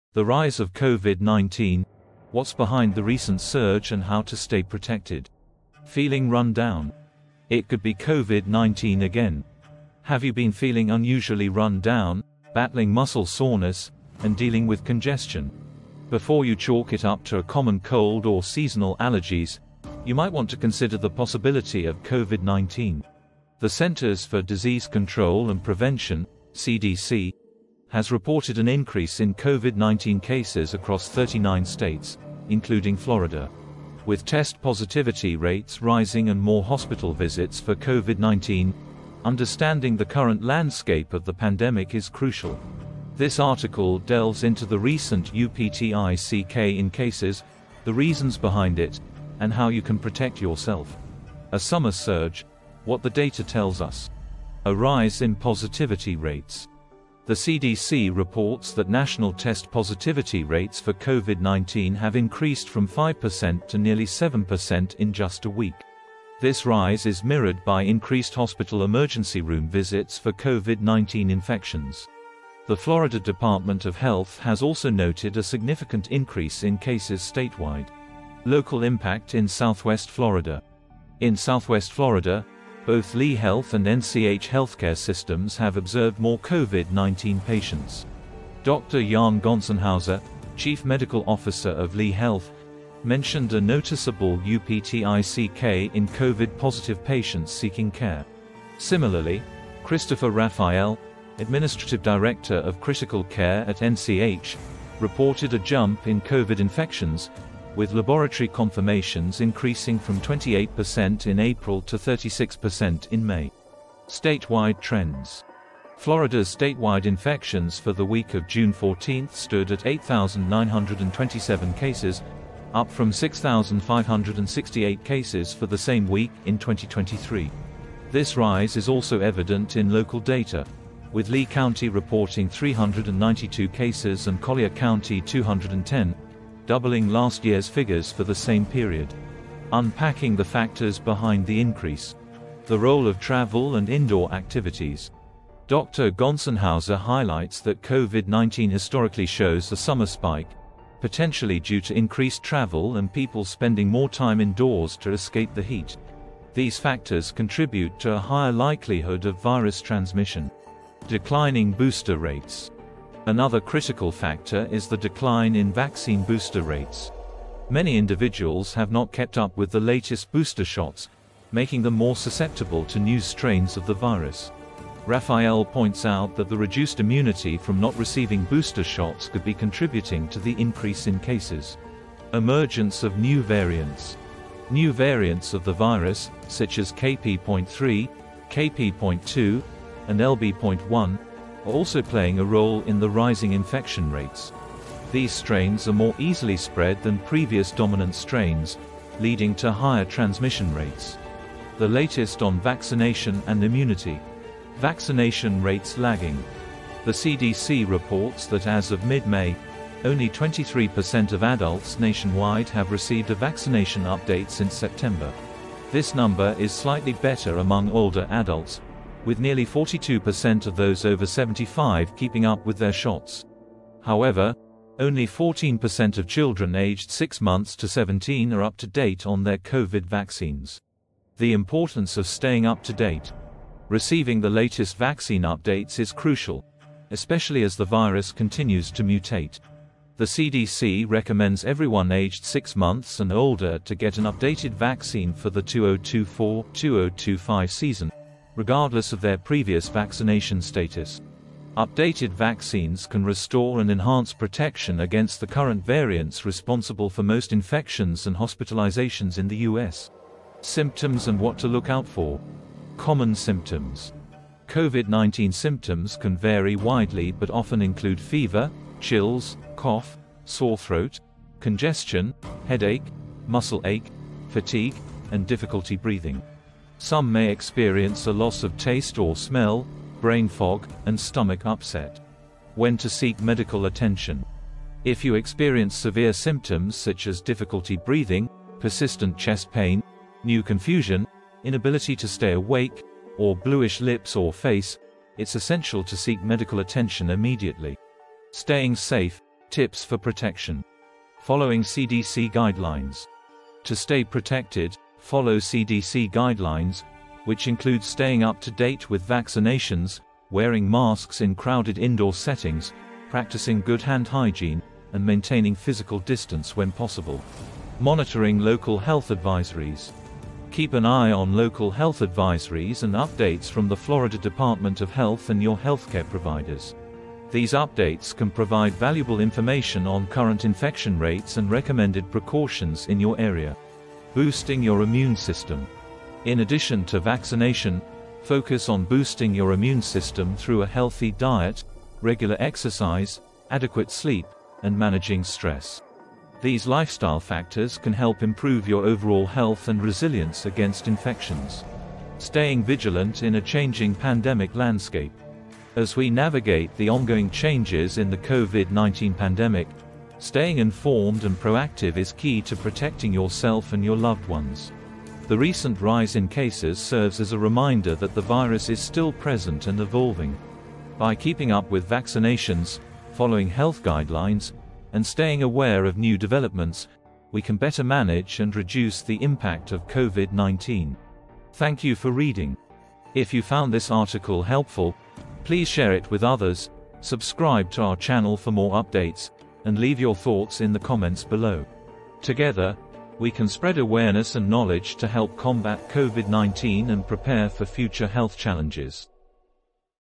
COVID-19 Uptick in Florida: What You Need to Know
A Familiar Foe Resurfaces

Feeling run-down with muscle soreness and congestion? Before dismissing it as a seasonal bug, consider this: COVID-19 is on the rise again. According to the Centers for Disease Control and Prevention (CDC), COVID-19 cases are increasing in 39 states, including Florida. As test positivity rates and hospital visits climb, it’s time to take a closer look at what’s happening and how you can stay protected.

## Chapter 1: The Current COVID-19 Landscape

### A Surge in Cases Nationwide

COVID-19 test positivity rates have surged nationally, jumping from 5% to nearly 7% in a week. This uptick is mirrored by increased emergency room visits for COVID infections. While these numbers are not yet at alarming levels, they indicate a trend that could worsen as the summer progresses. The CDC’s data shows a consistent rise in infections across many states, raising concerns about another potential wave.

### Florida's Rising Numbers

In Florida, the situation is becoming more pronounced. The Florida Department of Health reported 8,927 cases in the week of June 14, up from 6,568 cases the same week in 2023. Lee County saw 392 cases, and Collier County had 210, both doubling from the previous year’s figures. This increase in cases is being monitored closely by health professionals, who are seeing a corresponding rise in patients seeking care.

## Chapter 2: Local Impact - What Florida Hospitals Are Seeing

### Observations from Lee Health and NCH

Healthcare systems in Southwest Florida are feeling the impact. Dr. Iahn Gonsenhauser, chief medical officer at Lee Health, noted a small but noticeable uptick in COVID-positive patients. Similarly, NCH, a private nonprofit system in Collier County, reported a rise in COVID infections, with 36% of all laboratory confirmations in May, up from 28% in April. This trend could continue through the summer, as more people travel and spend time indoors to escape the heat.

### Hospital Preparedness

Both Lee Health and NCH are prepared to handle the increase in cases. They have implemented measures to ensure adequate staffing and resources to treat COVID-19 patients. These preparations are crucial in preventing healthcare systems from becoming overwhelmed, as seen in previous waves of the pandemic.

## Chapter 3: Understanding the Factors Behind the Rise

### New Variants and Their Impact

New variants of the virus are contributing to the rise in cases. The CDC reports that the KP.3 strain accounts for 33% of infections nationwide, followed by the KP.2 strain at nearly 21%, and the LB.1 strain at 18%. These variants are more easily spread than previous strains, which explains the rapid increase in cases.

### Decline in Vaccination Rates

Another factor is the decline in vaccination booster rates. Dr. Gonsenhauser noted that booster rates have significantly decreased, making it easier for the virus to spread. The combination of more transmissible variants and lower vaccination rates creates a perfect storm for increasing infections.

## Chapter 4: Staying Protected - The Role of Vaccinations

### The Importance of Staying Up-to-Date

Vaccinations remain a critical tool in combating COVID-19. The CDC recommends that everyone aged 6 months and older receive an updated vaccine, regardless of previous vaccination status. This updated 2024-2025 COVID-19 vaccine is designed to enhance protection against the current variants causing most infections and hospitalizations.

### Current Vaccination Statistics

## Chapter 5: Recognizing Symptoms and Taking Action

### Common Symptoms to Watch For

COVID-19 symptoms can include fever or chills, cough, sore throat, congestion, headache, muscle ache, fatigue, and difficulty breathing. Some individuals may experience a loss of taste or smell, brain fog, and stomach upset. Recognizing these symptoms early can help in seeking timely medical care and preventing further spread.

### What to Do If You Have Symptoms

If you experience any of these symptoms, it’s important to get tested for COVID-19. Early detection can lead to better management of the illness and reduce the risk of spreading it to others. Additionally, staying informed about local health guidelines and updates can help you take appropriate precautions.

As COVID-19 cases rise again, staying vigilant is crucial. By understanding the factors behind the increase, keeping up with vaccinations, and recognizing symptoms early, we can collectively mitigate the impact of the virus. Remember, the situation can change rapidly, so staying informed and prepared is key.

Thank you for reading! Together, we can navigate these challenging times and stay safe.